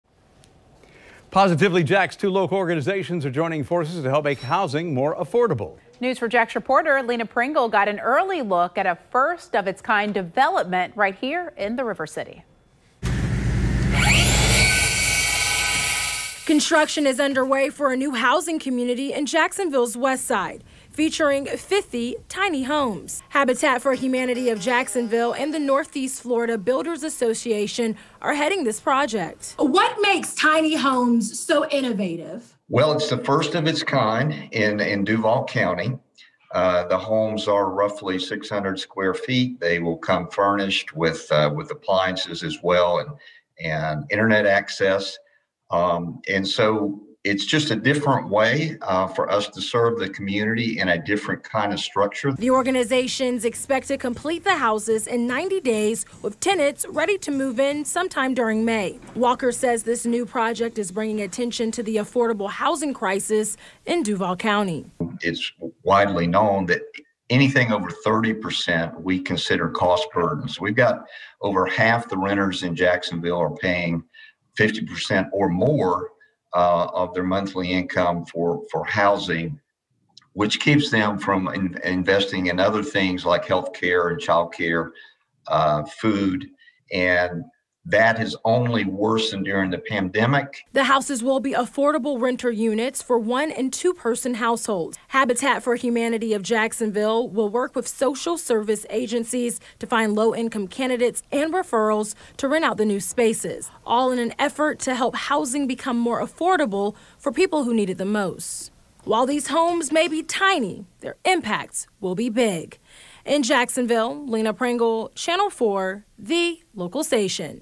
Habijax Homes
Due to the pandemic, many people are finding it hard to keep a roof over their heads. Two local organizations, Habitat for Humanity of Jacksonville and the Northeast Florida Builders Association, have teamed up to make affordable housing for those who are in need.